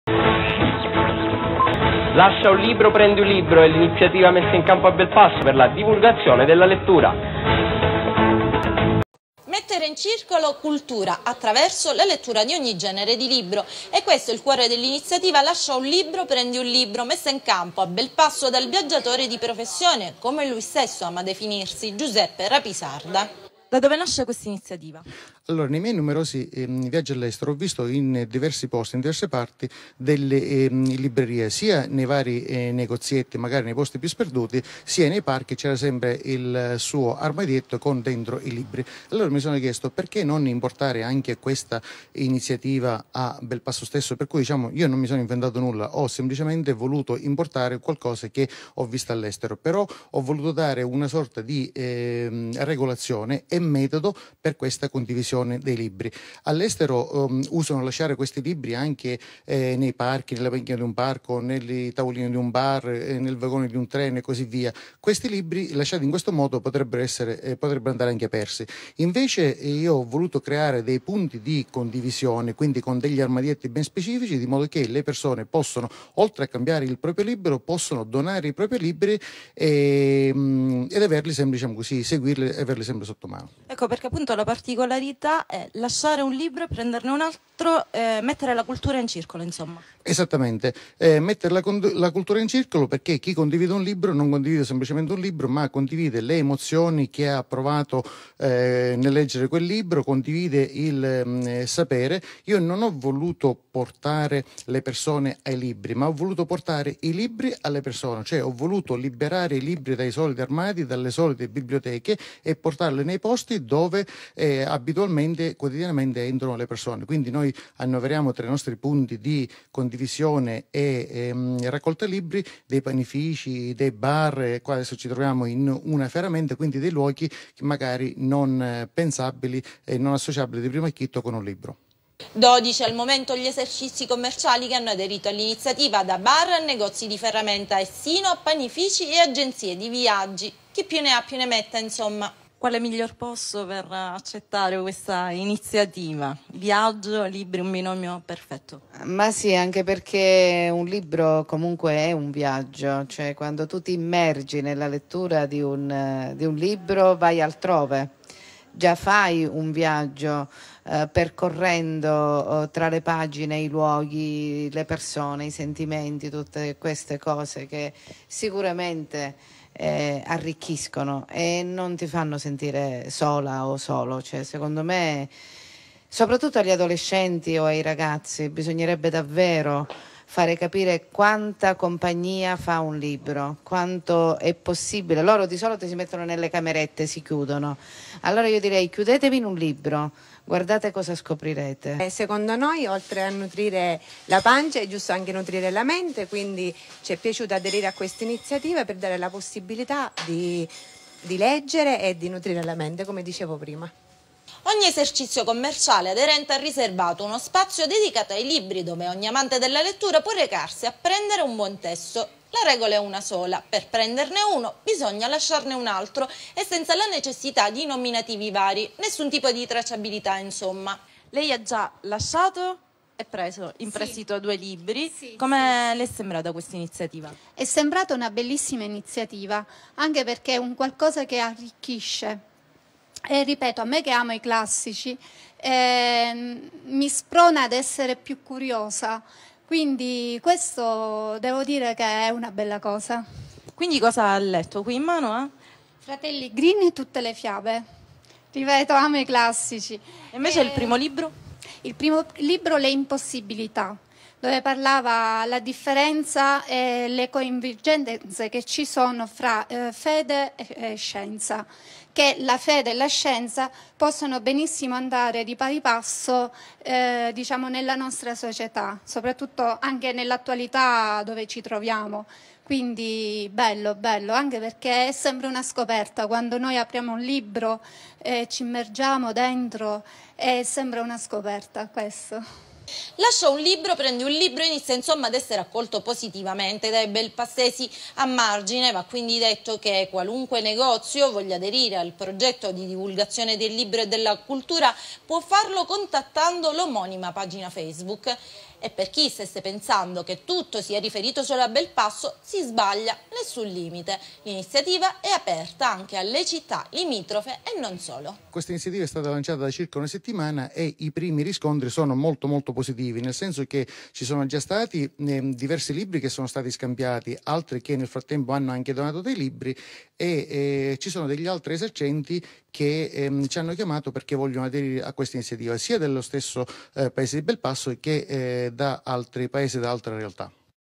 CONDIVISIONE GRATUITA DEI LIBRI: "Lascia un libro, prendi un libro"
Progetto di condivisone gratuita dei libri, partito dalla città di Belpasso (CT) ed ideato da Giuseppe Rapisarda.